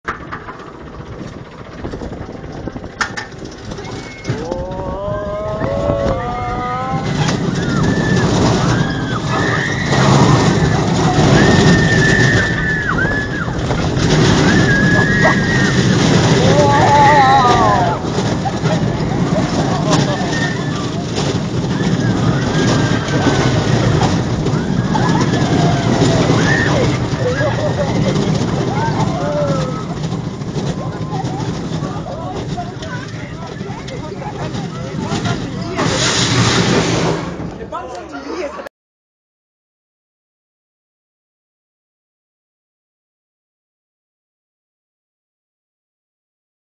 Ride the Shockwave rollercoaster at Drayton Manor theme park
A video I took whilst on the Shockwave stand-up rollercoaster at Drayton Manor theme park, in Staffordshire, UK.